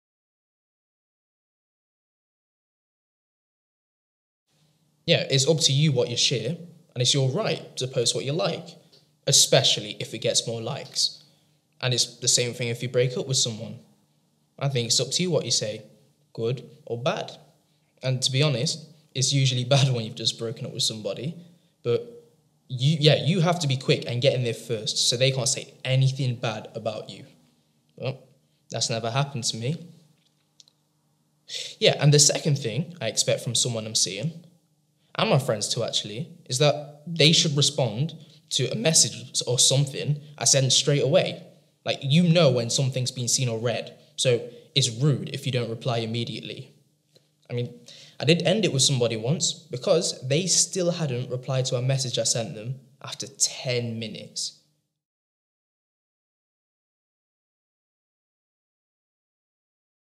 Myth vs Reality PSHE Toolkit: Healthy Relationships - Ryan "It's what I expect."
These talking heads have been formed from opinions and information we gathered through a series of focus groups conducted across the UK with young people aged 11-16. Each talking head represents a varied viewpoint about online experiences, considering if we can trust everything we see online and how you separate the myths from the reality.

You will find an outline of how to use the talking heads along with follow on discussion questions within the accompanying lesson plans available at: